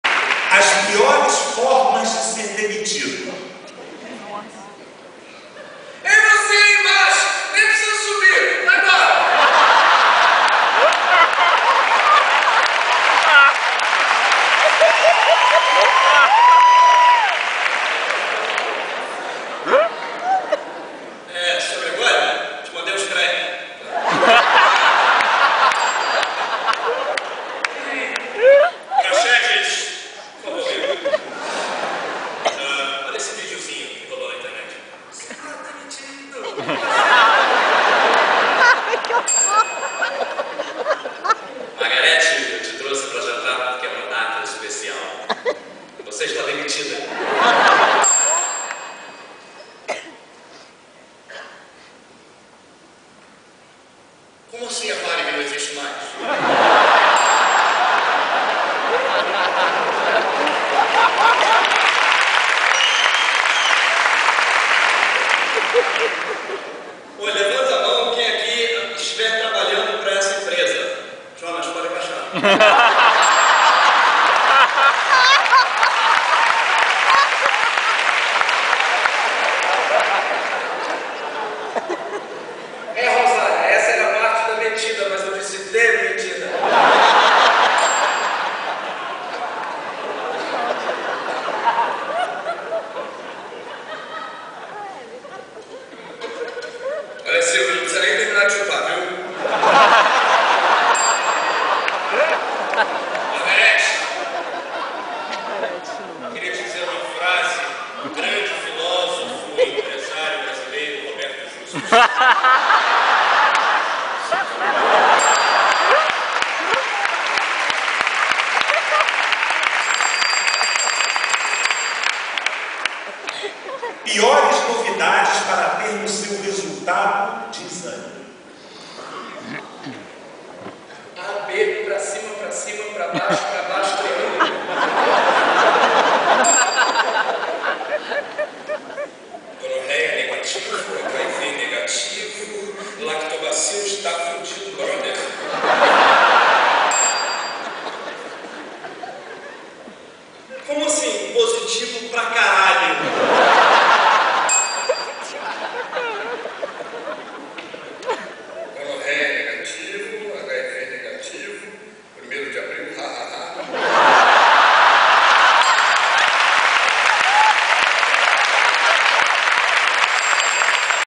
zenas emprovisadas: desafios - parte 3 (05/04/11)
05/04/11: 19 ª temporada - diretor: nizo neto & ator: bruno mazzeo / desafios / vivo rio (centro / rio de janeiro)